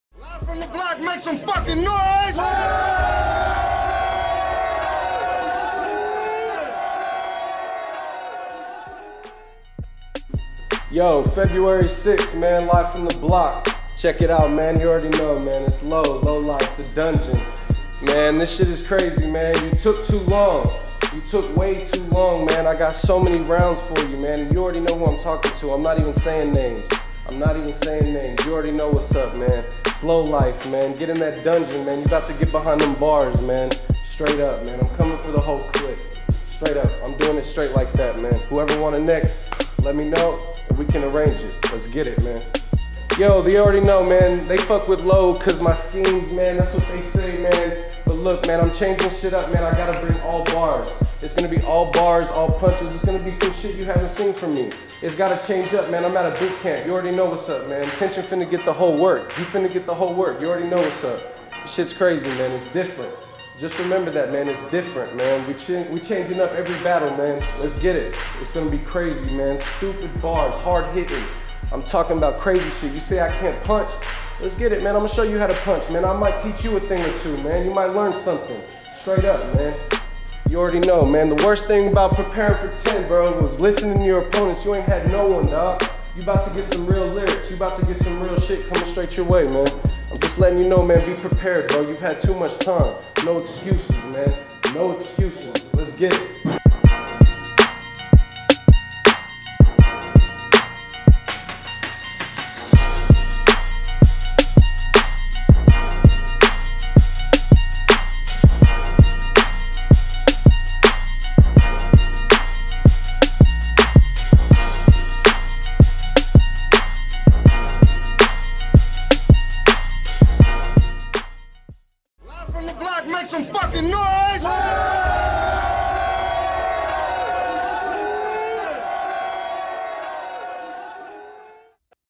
LO - speaks on Lo vs Tenchun
Lo speaks on his battle with tenchun feb 6th @kushies dont miss this vent hosted by the one and only POISON PEN! Here's the link to buy your tickets

Check out "LFTB BATTLES "ROTTING BEHIND BARZ 2" HOSTED BY POISON PEN" on Eventbrite!

Date: Sat, Feb 6

Location: 6720 Brentwood Stair Rd